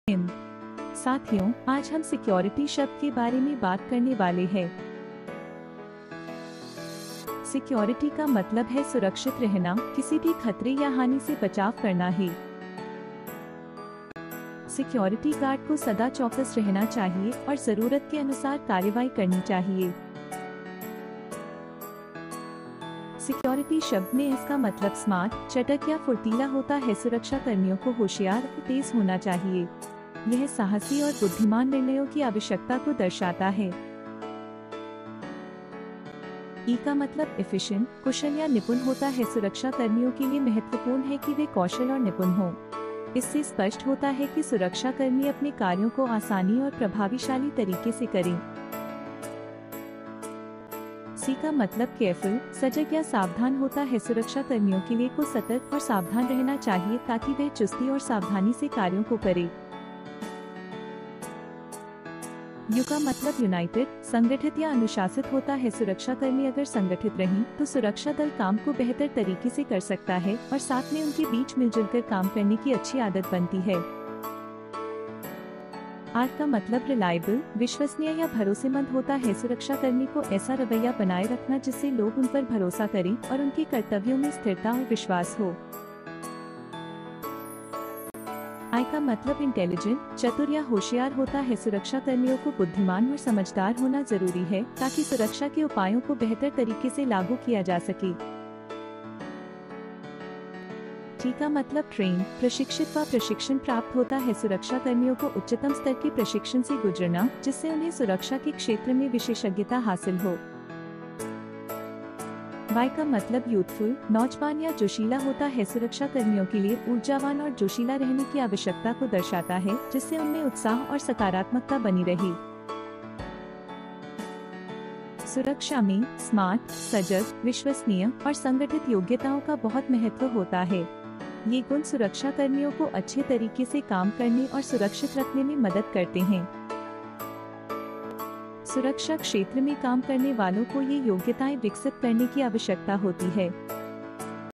security guard learning | security Shabd ka Matalab | security training
इस वीडियो में, हम जानेंगे कि सुरक्षा कर्मियों में कौन-कौन से गुण होने चाहिए जो उन्हें उनके काम में प्रभावी बनाते हैं। चाहे वो चालाक और सक्षम होना हो, या सावधान और एकजुट रहना हो, हर गुण सुरक्षा और सुरक्षितता की मदद करता है। हम देखेंगे कि प्रशिक्षित, विश्वासनीय, और बुद्धिमान सुरक्षा कर्मी कैसे सुरक्षित वातावरण बनाते हैं। हमारे साथ जुड़ें और समझें सफल सुरक्षा विशेषज्ञों को परिभाषित करने वाले महत्वपूर्ण गुण।